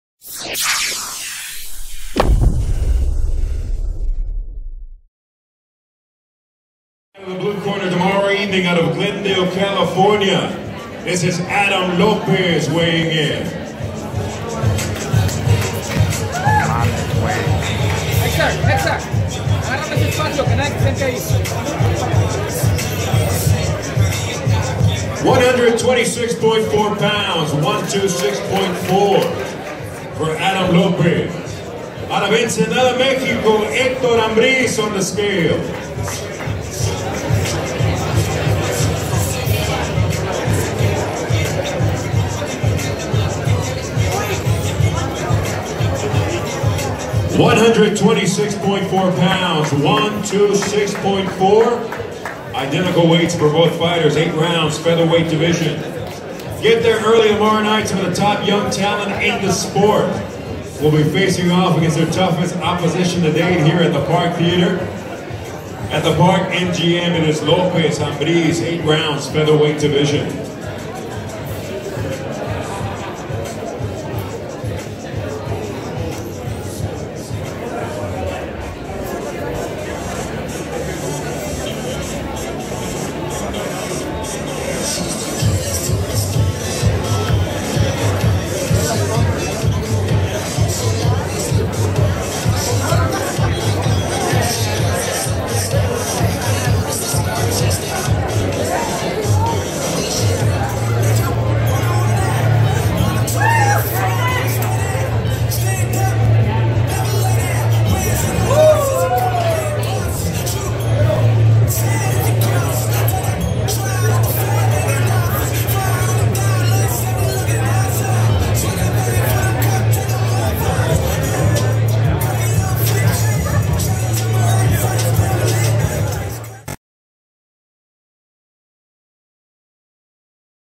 Adam Lopez (10-1, 4 KO's) vs Hector Ambriz (12-7-2, 6 KO's) Official Weigh-In
was on hand inside of the Park Theater in Las Vegas, NV as prospect Adam Lopez (10-1, 4 KO's) weighed in vs. Hector Ambriz (12-7-2, 6 KO's) on the Ryota Murata undercard on ESPN +.